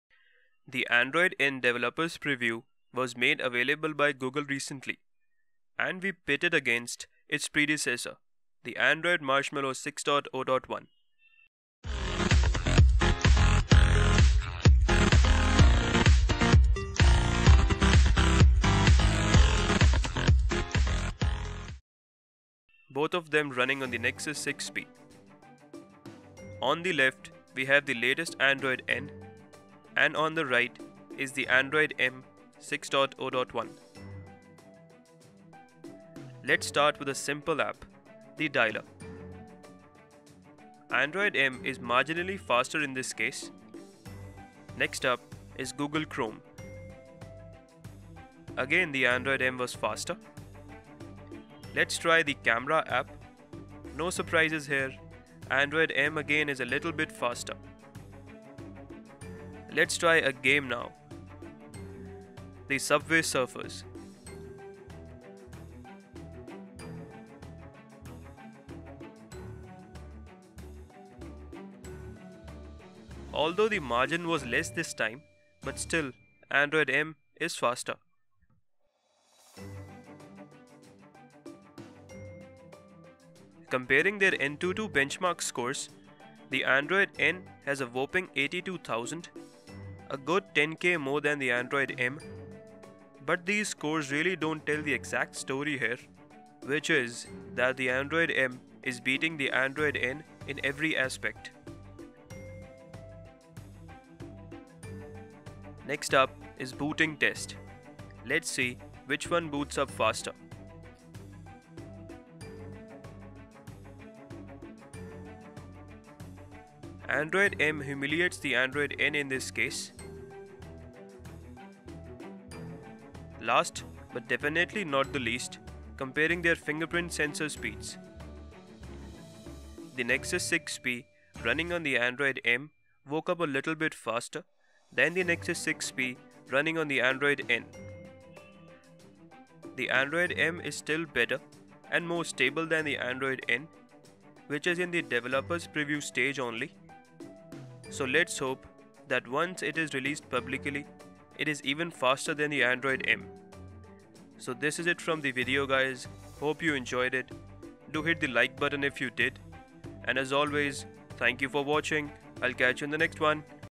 Android N VS Android M : Speed Test on Nexus 6P!
Android N VS Android M : Speed Test on Nexus 6P By TechTube!

Android N developers preview is now available for download and we take a look at some of its new features which include multi-window,night mode,new settings etc

Gadgets Courtesy : Global Gadgets , New Delhi.